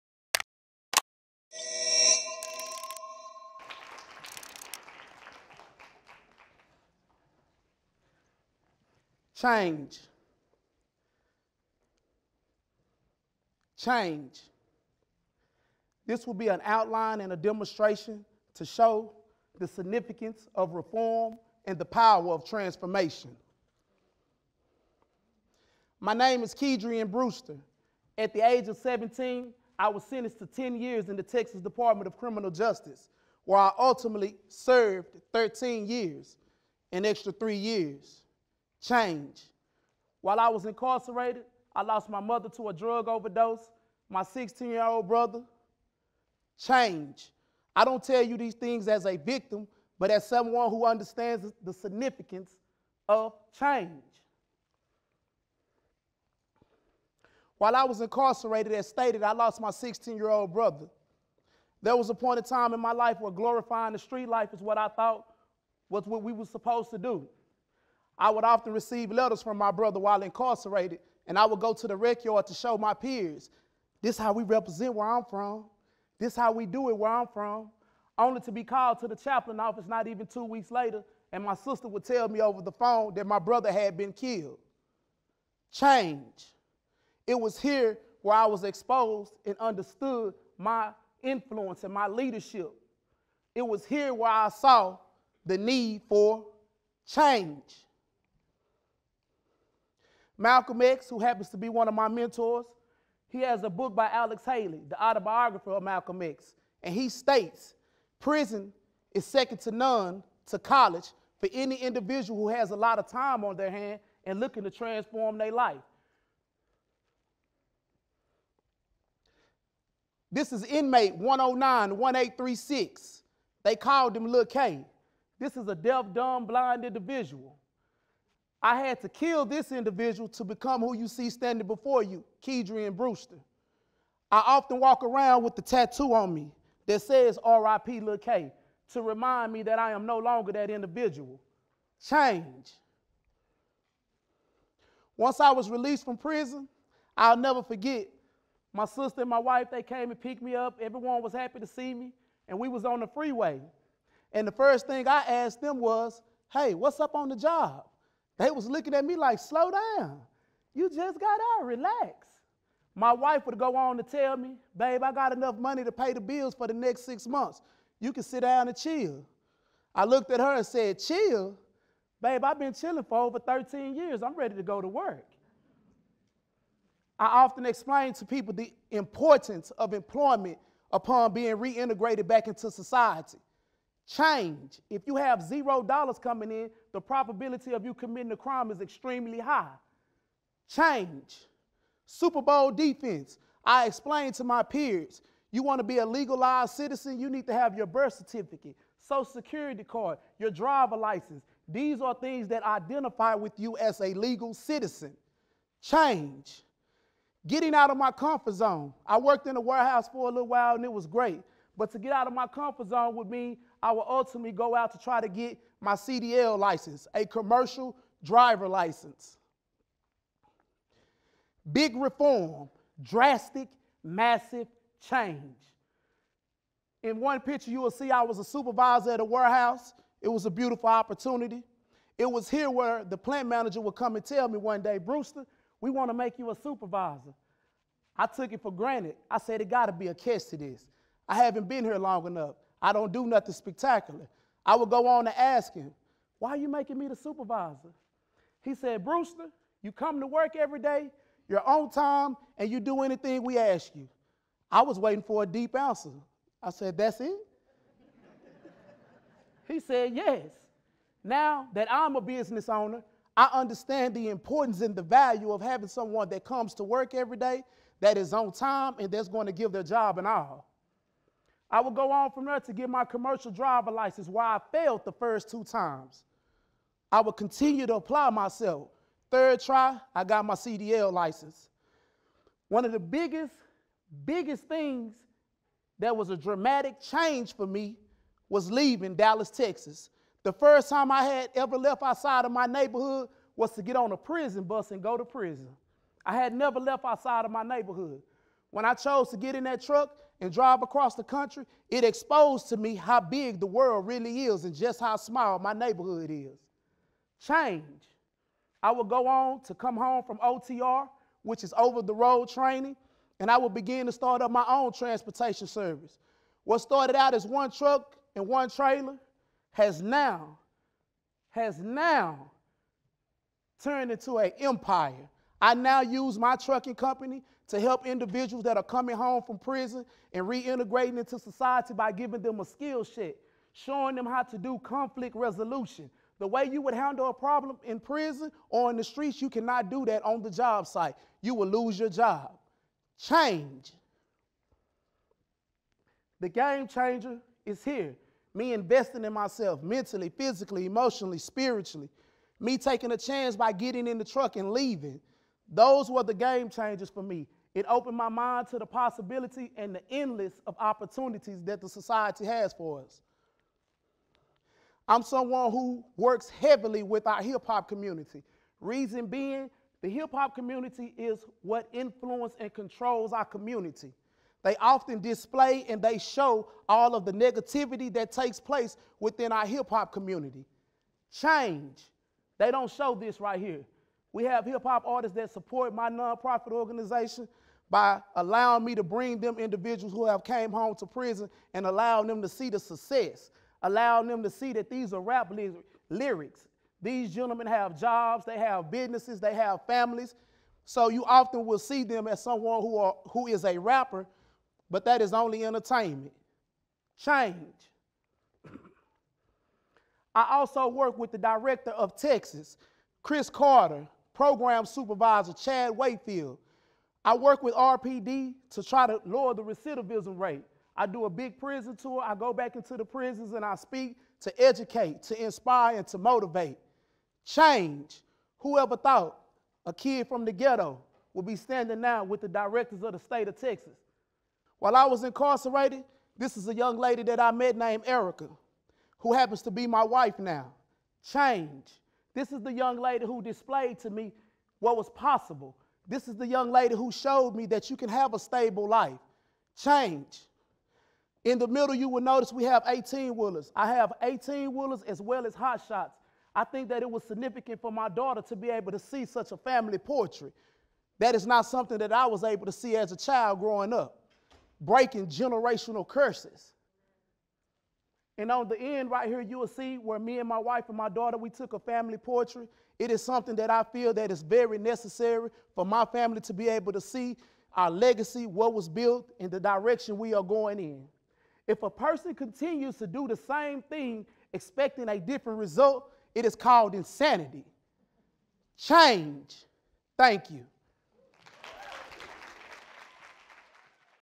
Big Reform | Keidrain Brewster | TEDxTCCD
I was completely lost and hopeless.
I was 17 years old when I went to prison to serve a 10-year sentence.
During my bid, I lost my grandmother that raised me, my mother, and my younger brother. That was a turning point to my transformation.

I remember telling the homies about my brother being down and the way he was "repping the hood". I had praised his actions.
This is when I realized that I had led my brother down the wrong path.
From this, the new "me" began to emerge. If I could lead someone to darkness, I could lead them to light.
Keidrain Brewster is a returned citizen that spent 13 years in the Texas prison system. Since his release, he has built his own trucking company & is the author of two books: From the Rec Yard to the Streets, which outlines lessons from his time in prison & how he applied them upon release; & From the Streets to the Suites, in which he tells of his journey starting his own business & reaching success. He also tours prisons with his inspiring message of reform and transformation. This talk was given at a TEDx event using the TED conference format but independently organized by a local community.